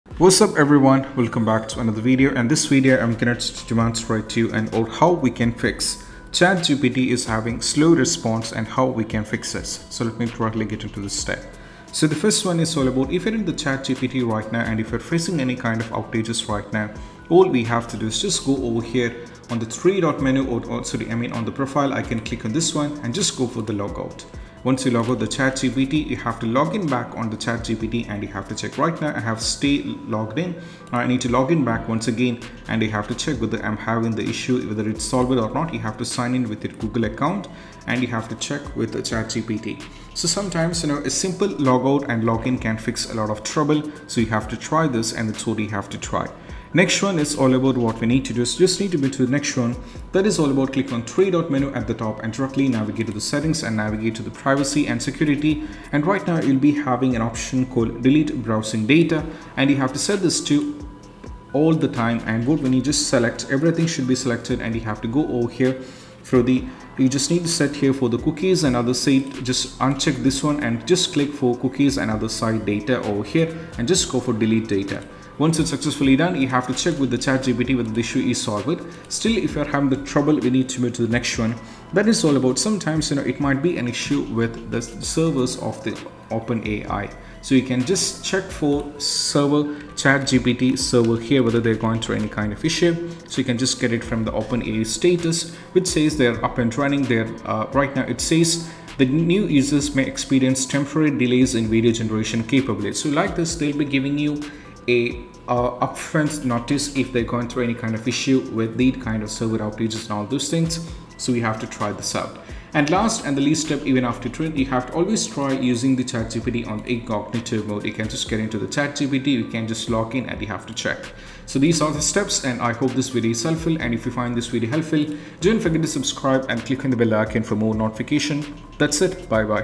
How To Fix ChatGPT Slow Responses (Guide)
Is ChatGPT taking too long to respond or generating answers slowly? In this guide, we'll walk you through simple steps to speed up ChatGPT's responses—like fixing network problems, clearing your browser cache, and signing out and back into your ChatGPT account.